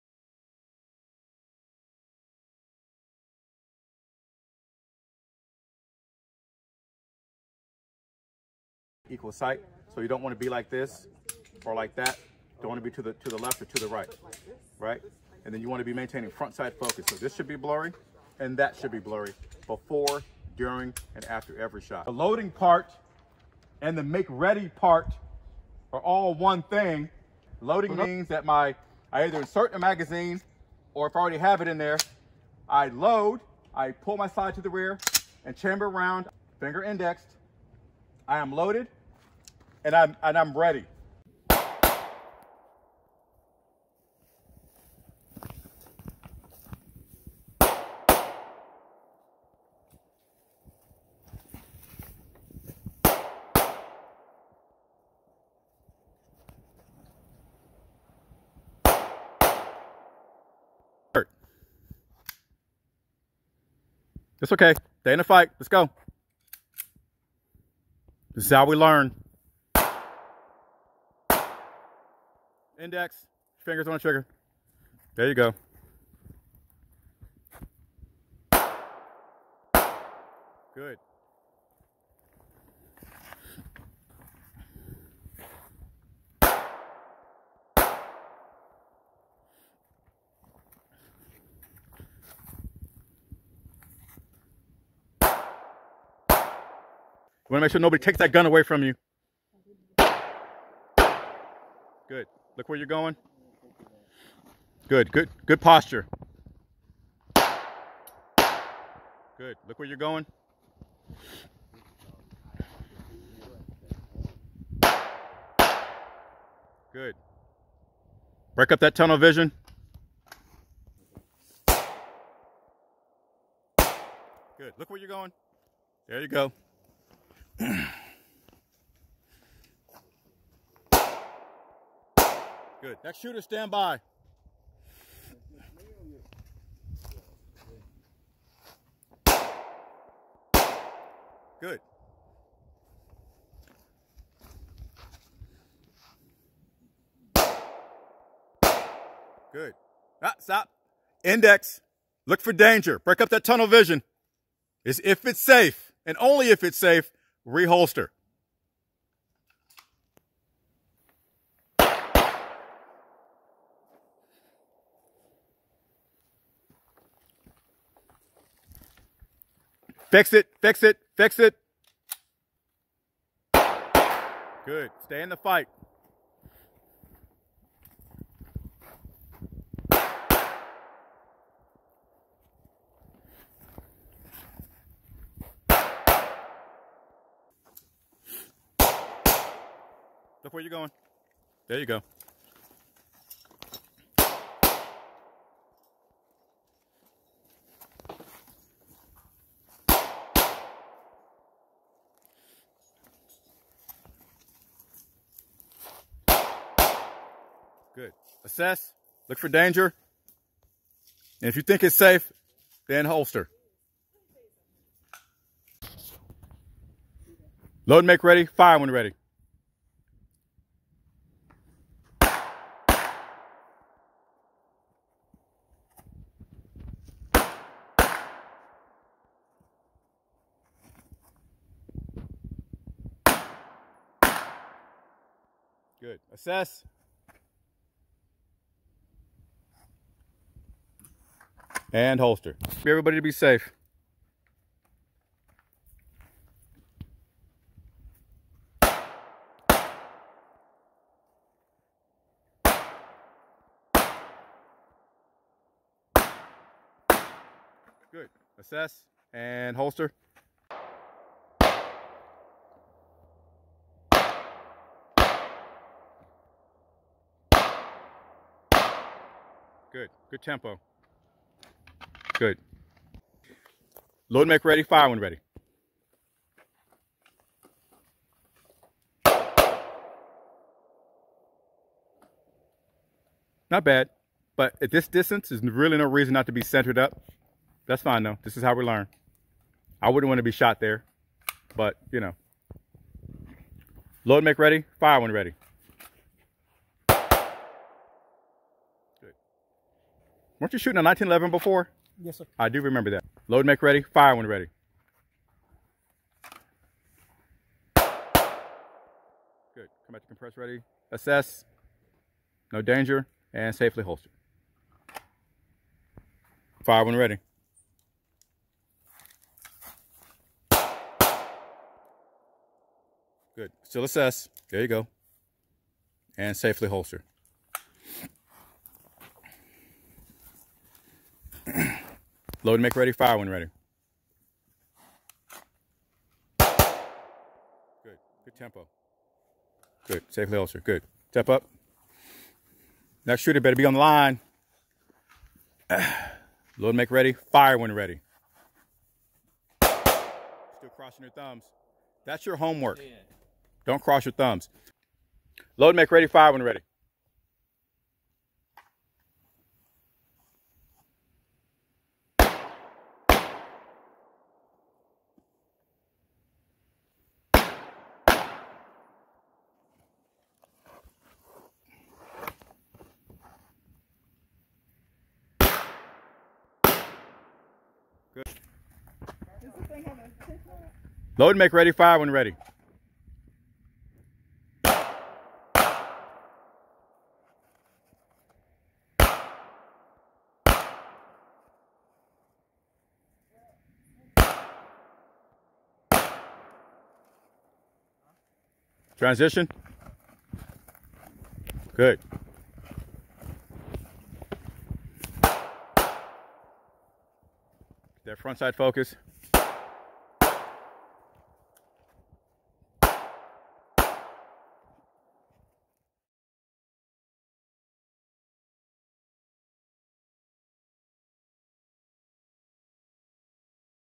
01MAY2021 Basic Pistol 101
Firearm Safety
Marksmanship
Load and Make Ready
Unload and Show Clear
Movement drills
VTAC drills